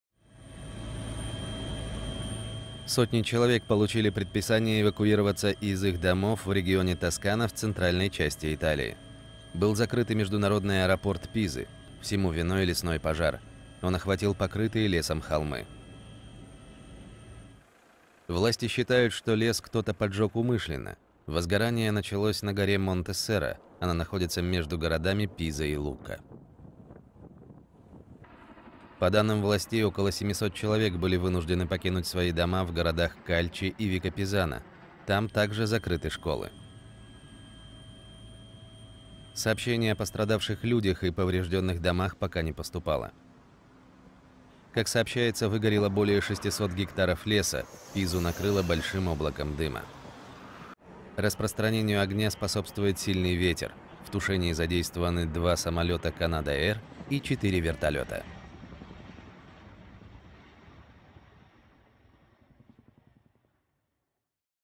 Сотни жителей Тосканы эвакуировали из-за лесного пожара
В понедельник в итальянской Тоскане начался лесной пожар, который охватил холмы и угрожает жилым домам. Сотни человек получили предписание эвакуироваться.